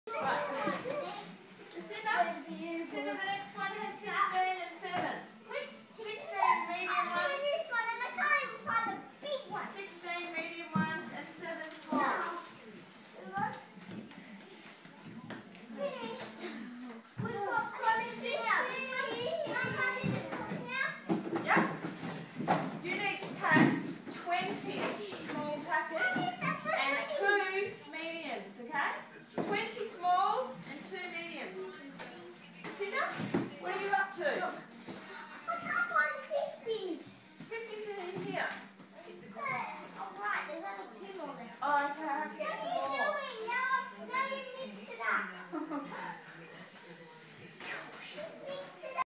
50 boxes to pack tonight so we've called out the re enforce
Lucinda and Zara stay up late to help pack boxes for the mornings mail. It's chaos, but the job get done!